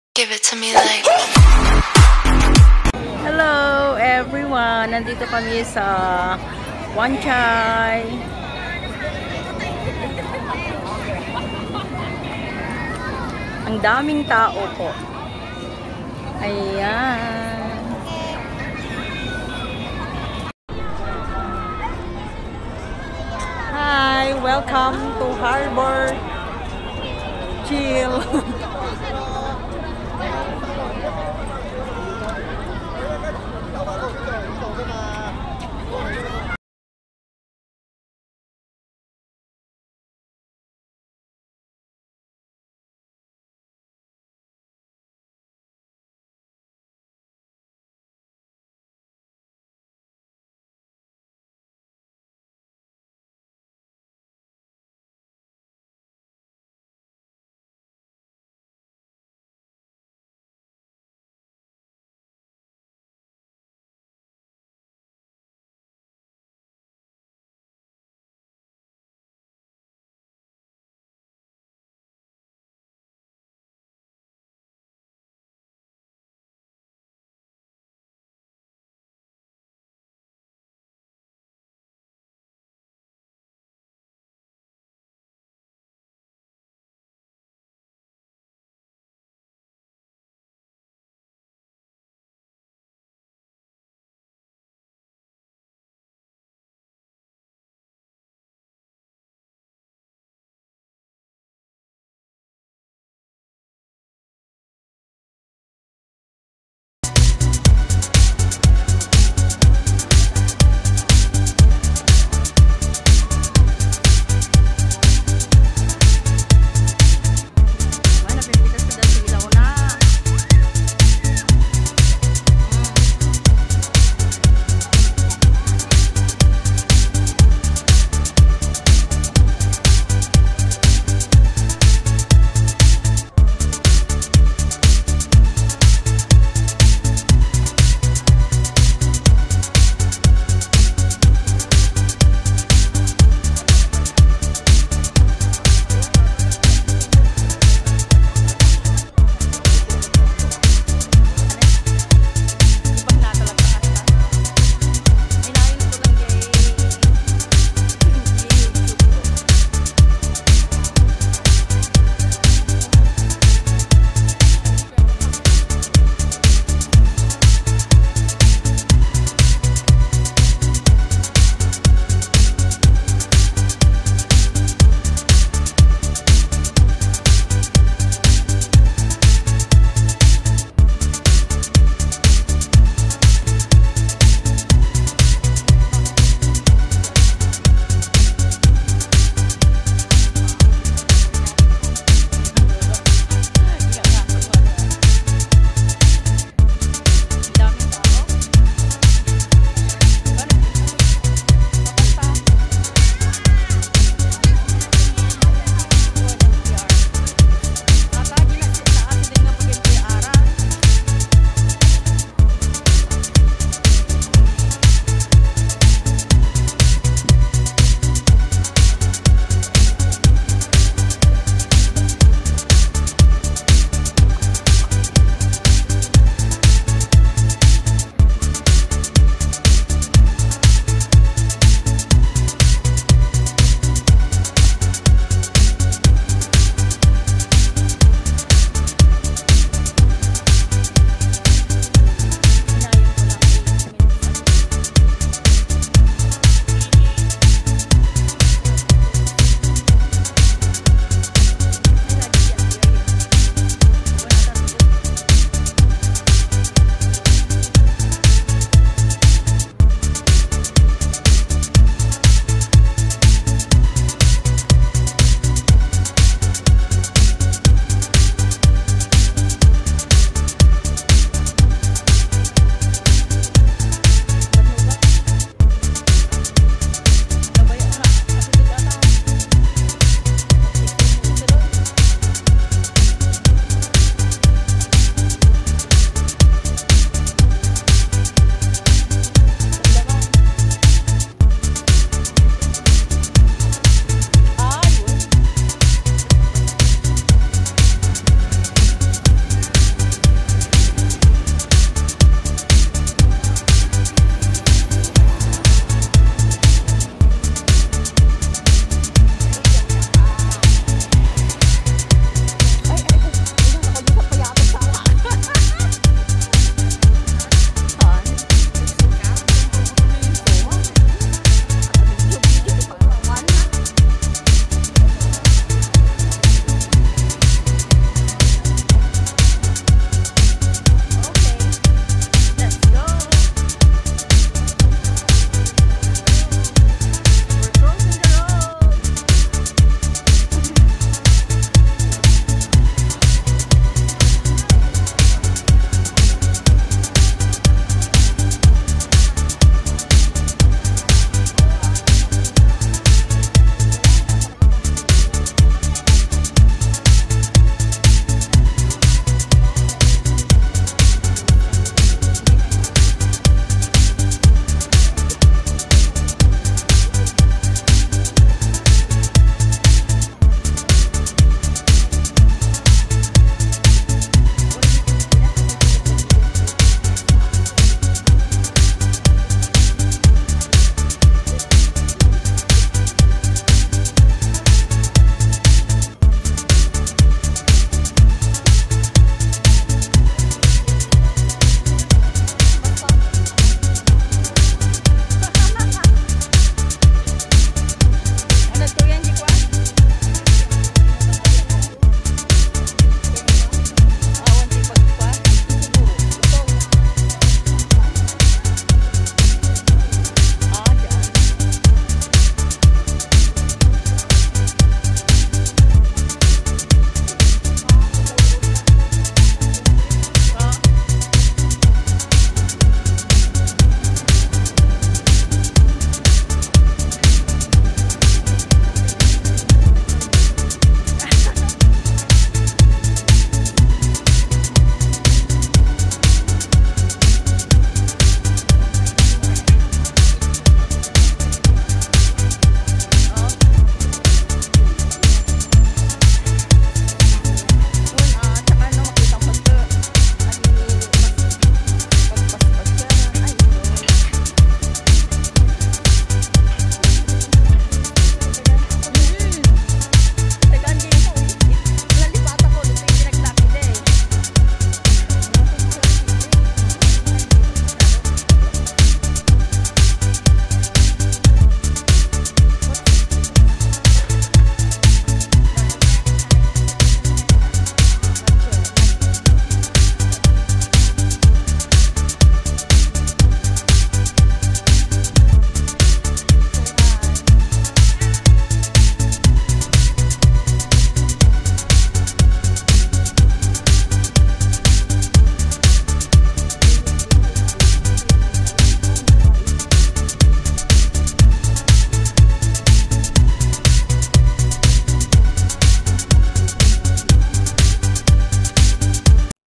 Happy Hallowen/Harbour @Momshee tv blog
A very nice display located at victoria harbour wanchai for holloween presenration.
come and let's take a look guys.